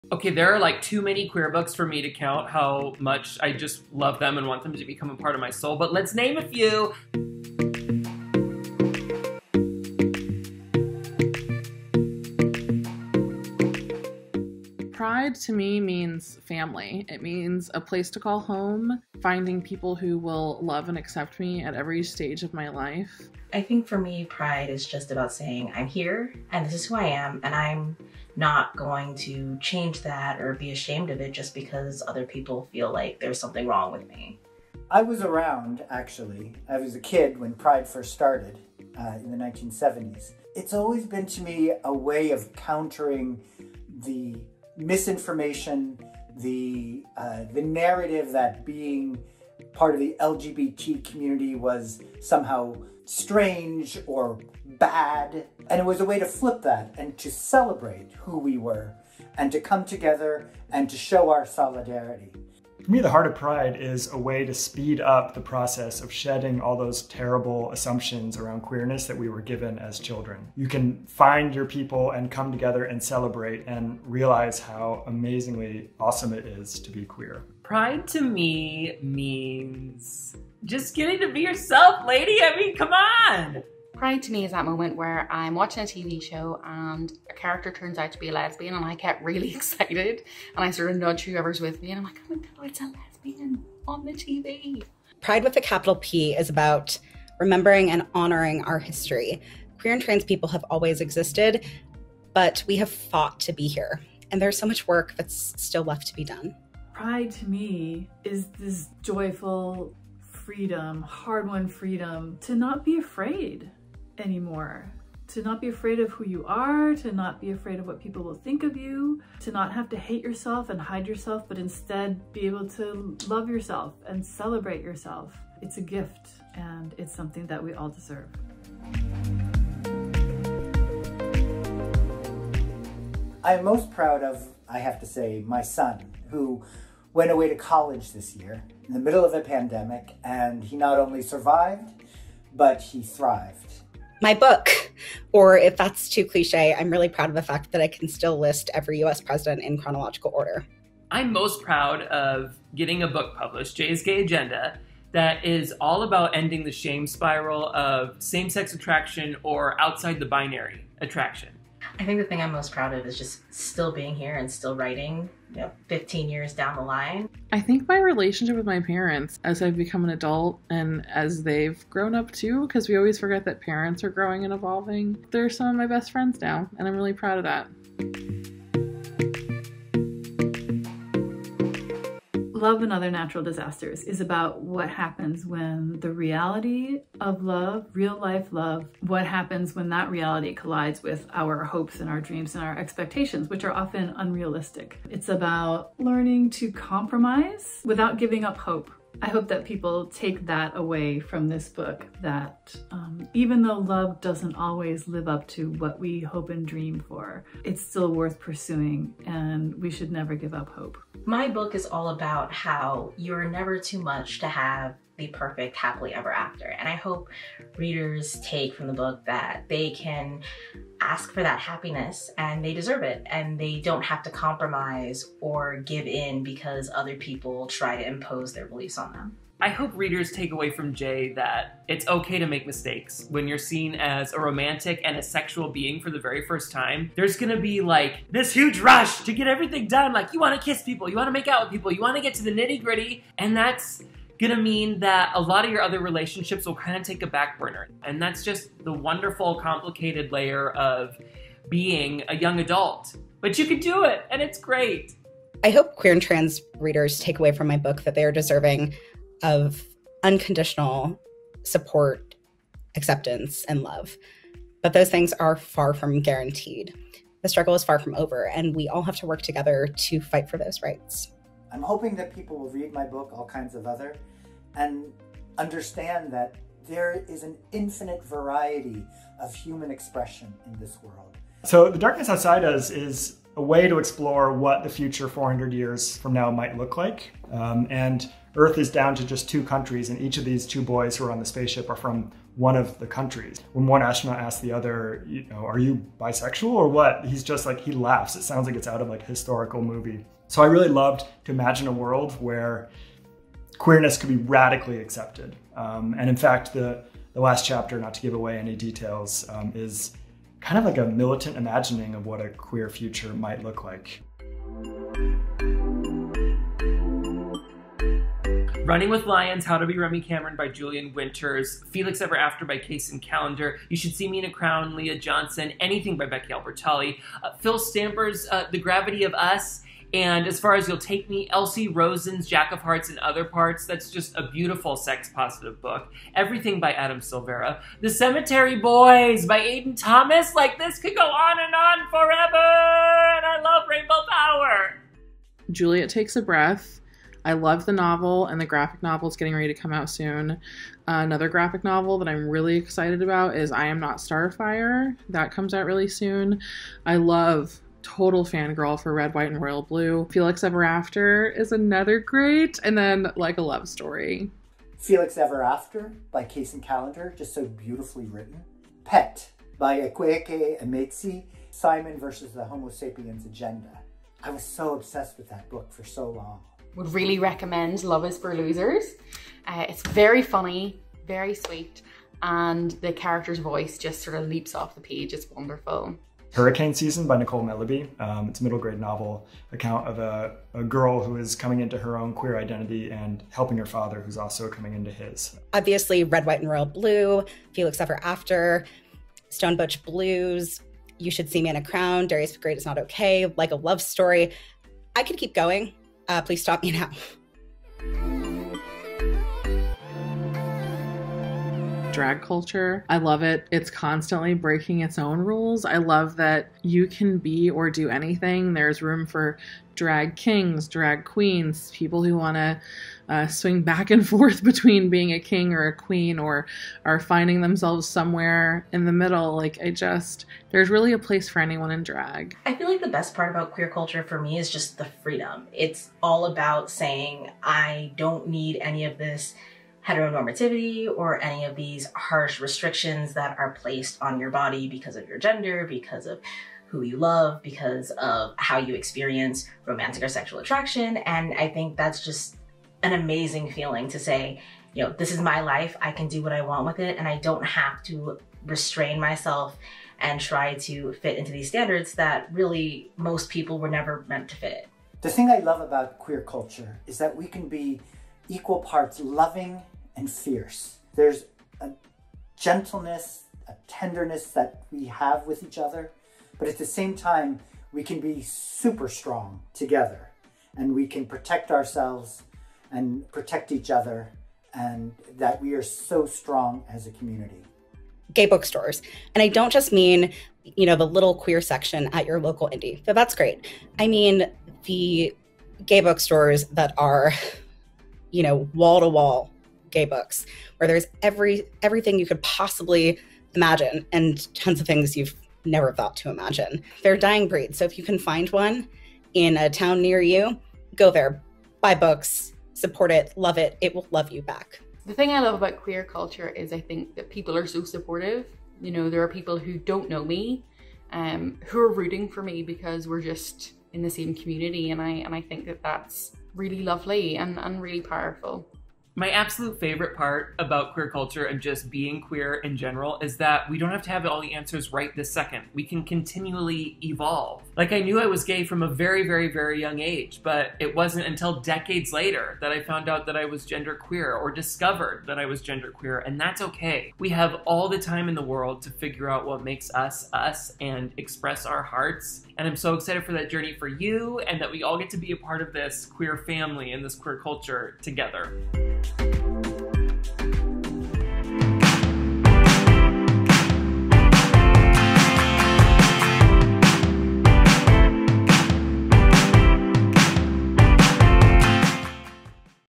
We’re Celebrating Epic Pride! 🏳️‍🌈Author Confessionals
What does Pride mean to some of our favorite authors? Celebrate Epic Pride this June (and all year round) with these amazing LGBTQIAP+ books and authors!

Whose answers do you relate to? Share them in the comments!